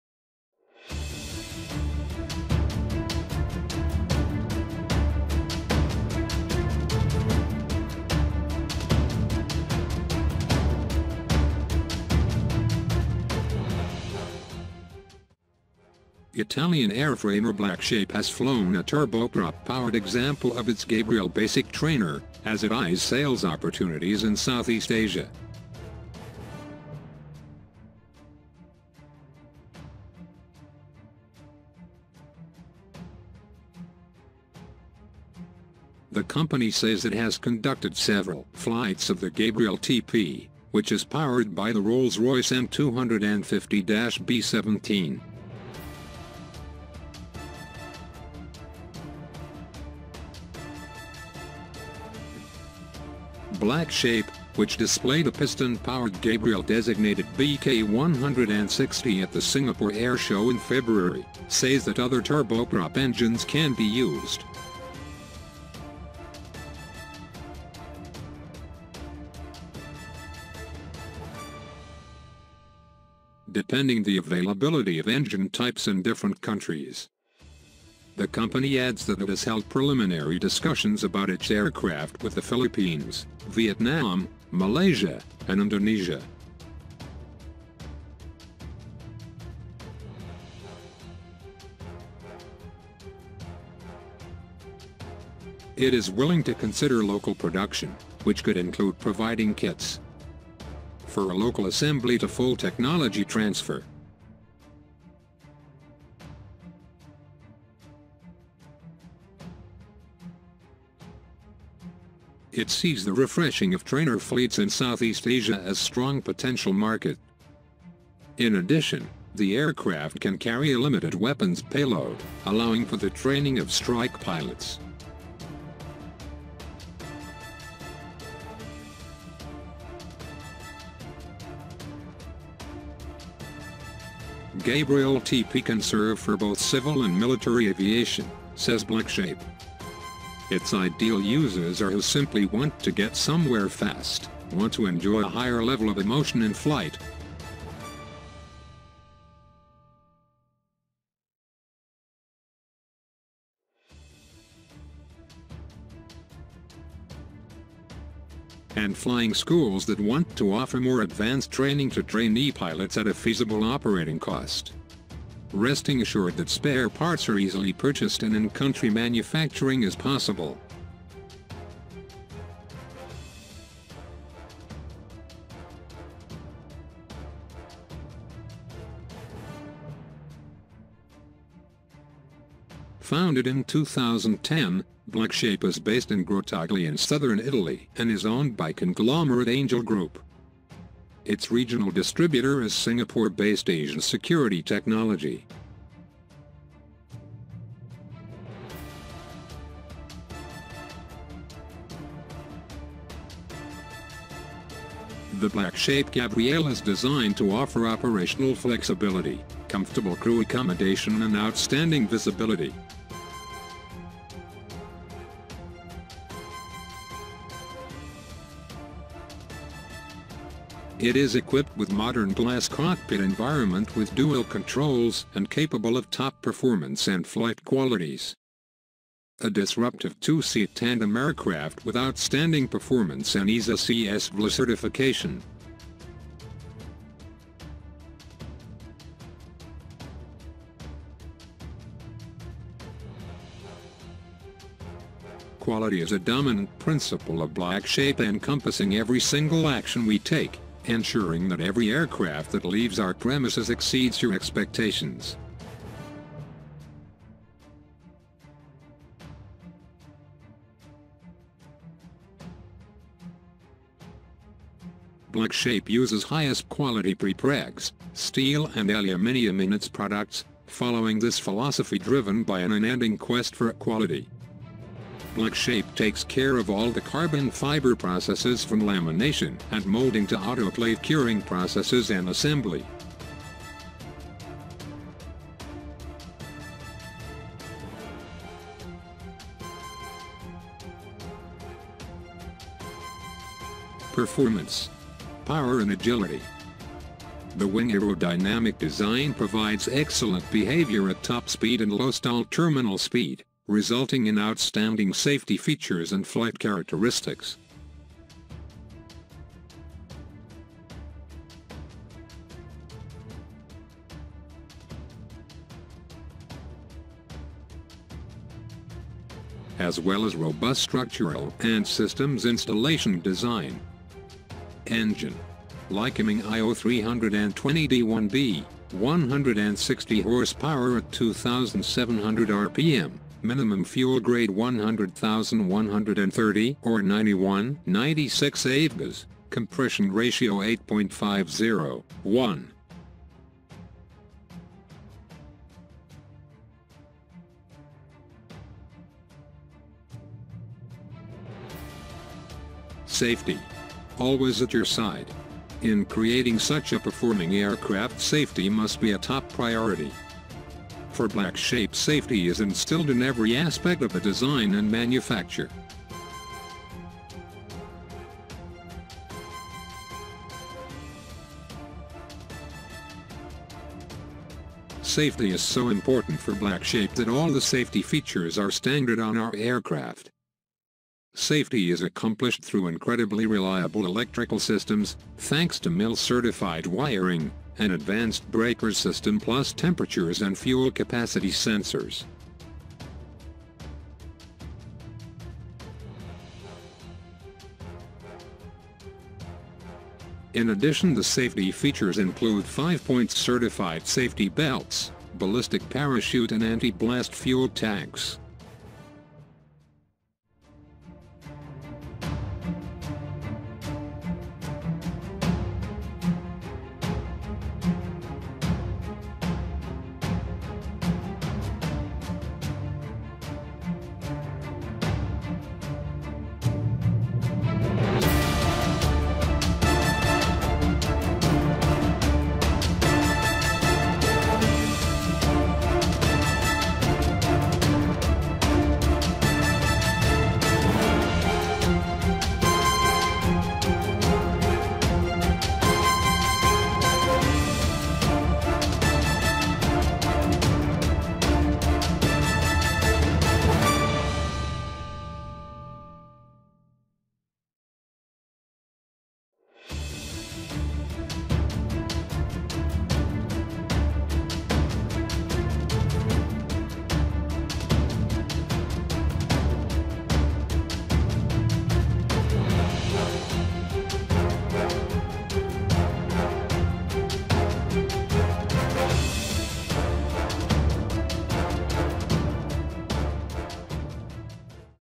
Italia Offering New Aircraft To Philippine Air Force | How sophisticated Italian Jet Aircraft?
Italian airframer Blackshape has flown a turboprop-powered example of its Gabriel basic trainer, as it eyes sales opportunities in Southeast Asia.

Blackshape, which displayed a piston-powered Gabriel designated BK-160 at the Singapore air show in February, says that other turboprop engines can be used, depending the availability of engine types in different countries.

The company adds that it has held preliminary discussions about its aircraft with the Philippines, Vietnam, Malaysia, and Indonesia. It is willing to consider local production, which could include providing kits
for local assembly to full technology transfer.